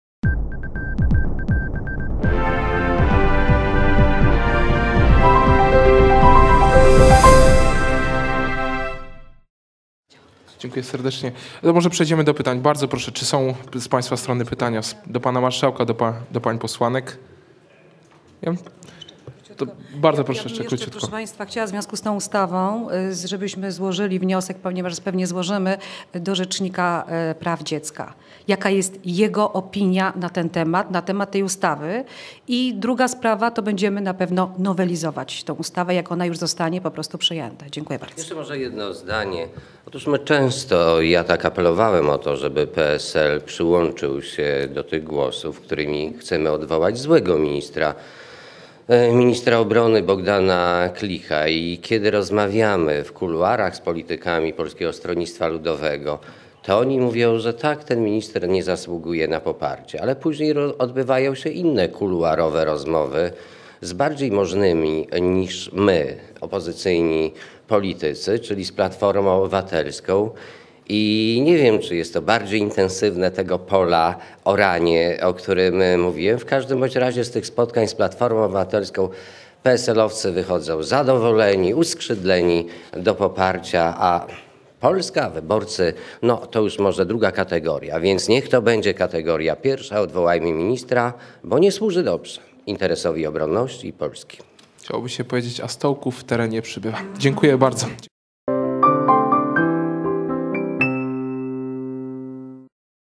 TVSLD (Konferencje) : Żłobki i apel do PSL - część 2
Na konferencji prasowej w sejmie SLD zaapelowało do PSL o nie popieranie ministra obrony narodowej Bogdana Klicha w głosowaniu nad wotum zaufania. Posłowie SLD odnieśli się również do ustawy o budowie żłobków i przedszkoli